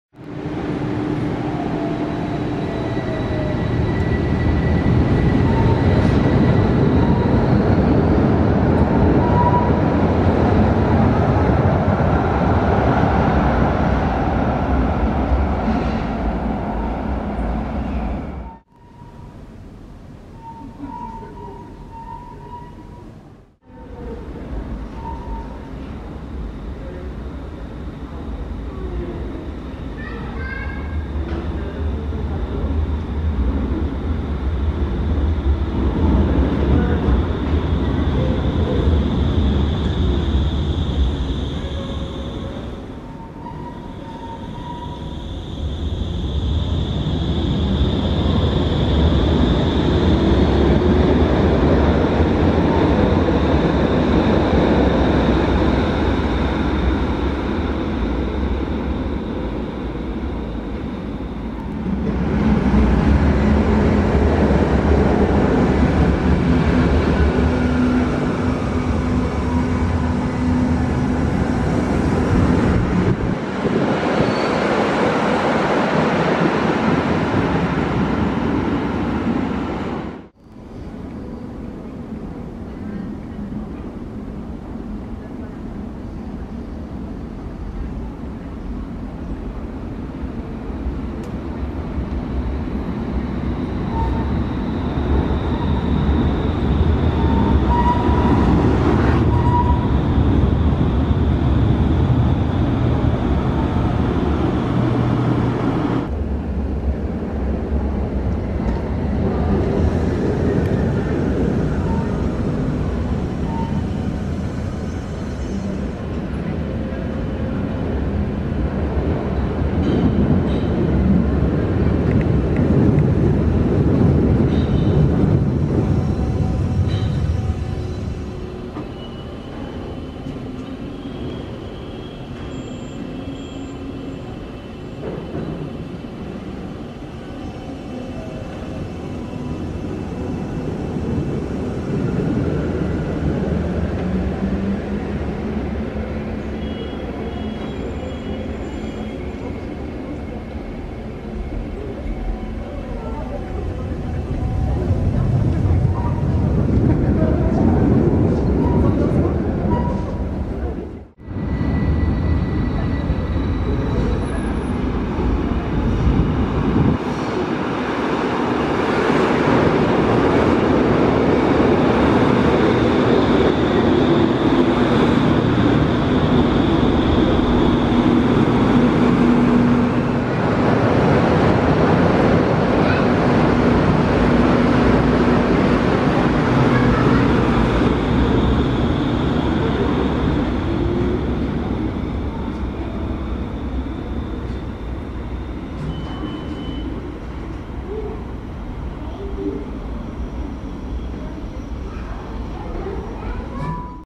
Stadtbahn Bielefeld (Light Rail) | Hauptbahnhof
German: In diesem Video seht ihr verschiedene Stadtbahnen, die es an der Haltestelle/Station Bielefeld Hauptbahnhof zu sehen gibt.

English: In this video you can see a variety of Stadtbahnen (a type of Tram/Metro) at Bielefeld main station. Bielefeld is a city with a population of 330,000 and lies in the north eastern part of the German state Nord Rhein-Westphalia (Nordrhein Westfalen).